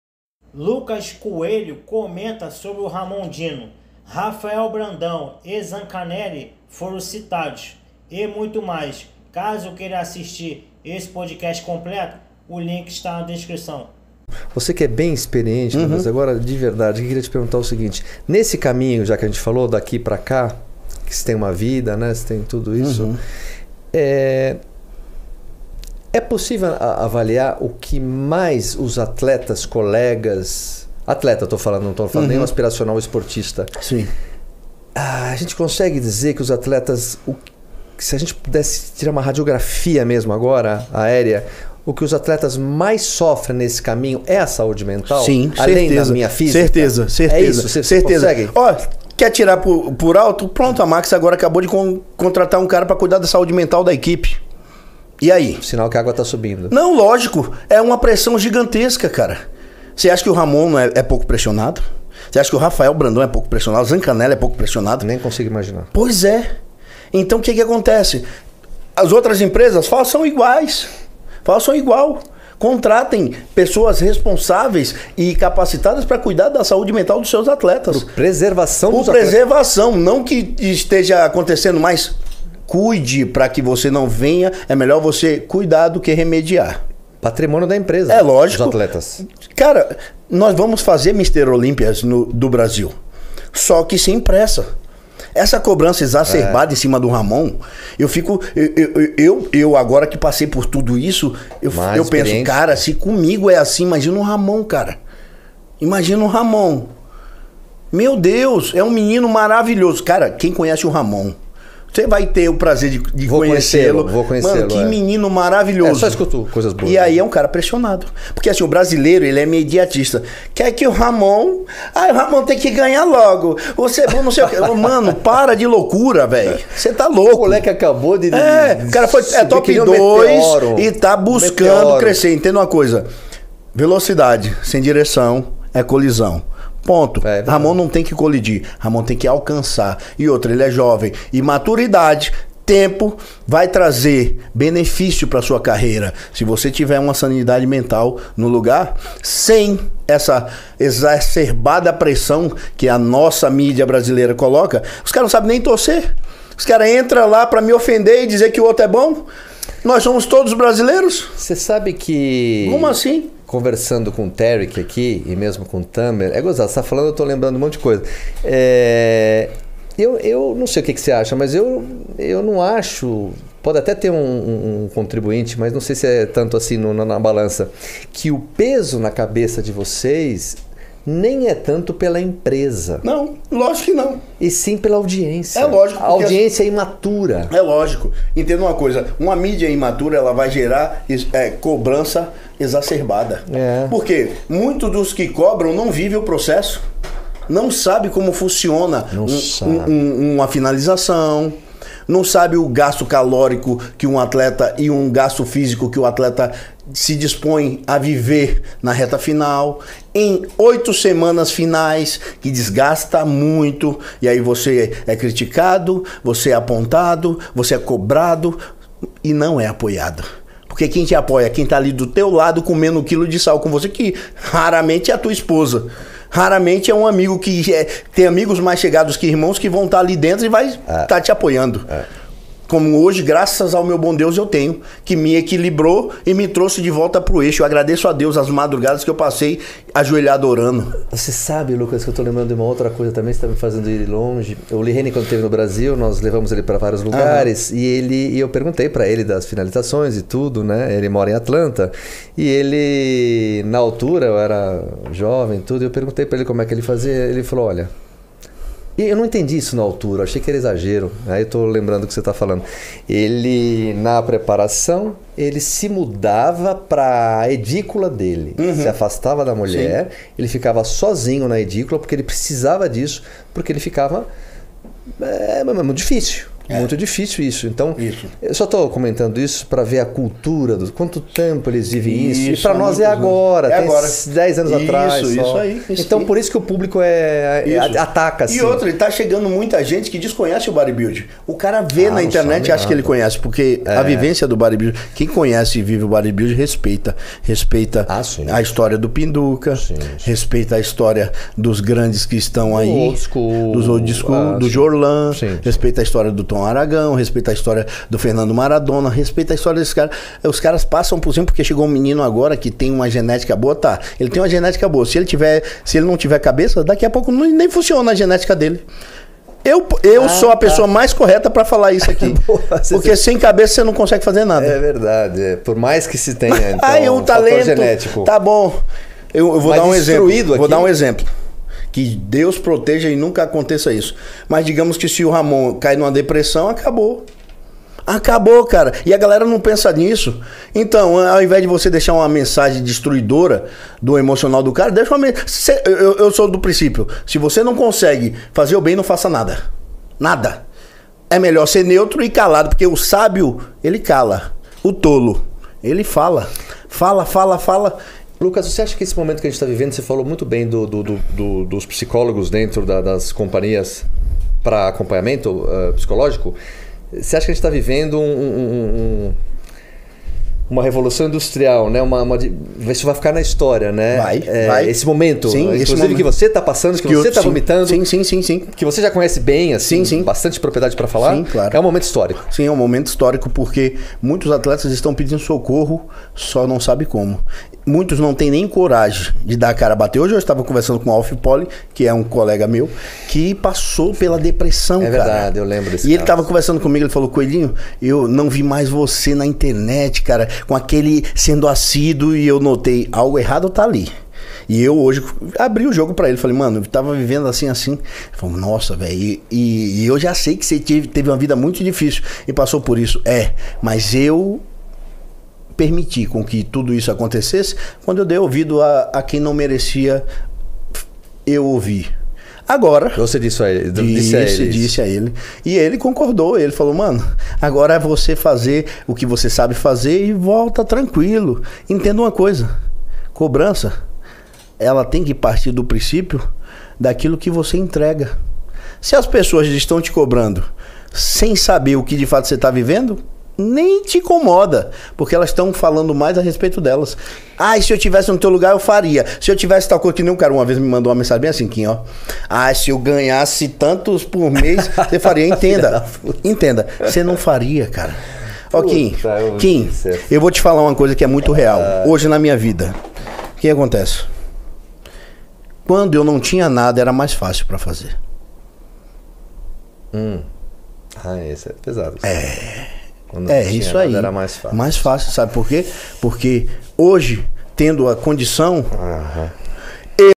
LUCAS A REAL sobre RAFAEL BRANDÃO ZANCANELLI RAMON DINO CITADOS NO PODCAST MARCELO QUINN
LINK DO PODCAST  COMPLETO ABAIXO !

Bem - vindo ao mundo  marombeiro
Vamos buscar trazer sempre tudo que acontece no mundo maromba